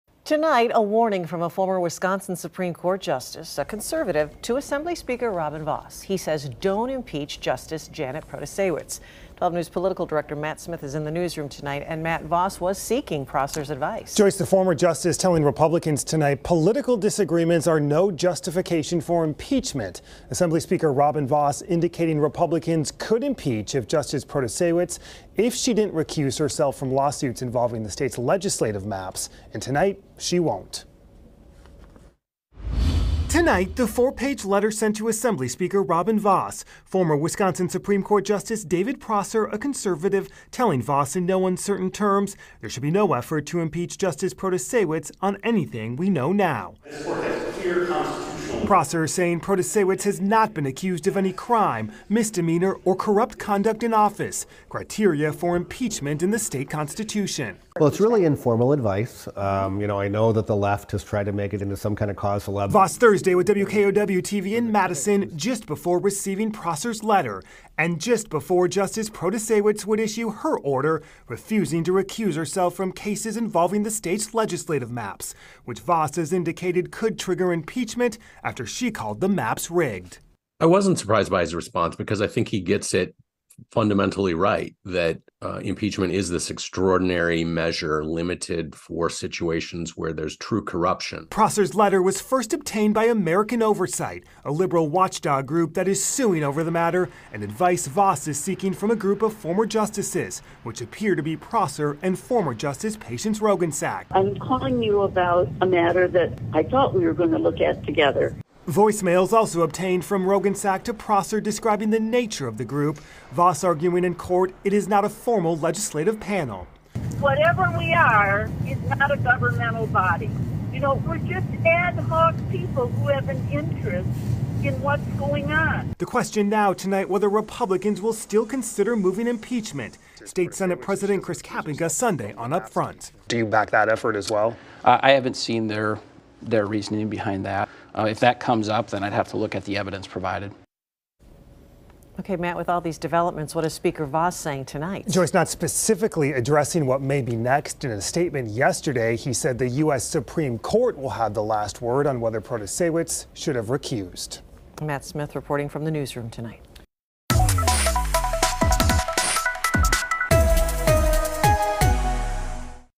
Former Wisconsin Supreme Court justice advises against impeaching Justice Protasiewicz
Former Wisconsin Supreme Court Justice David Prosser is advising Republicans not to impeach Justice Janet Protasiewicz after being asked to provide guidance by Assembly Speaker Robin Vos.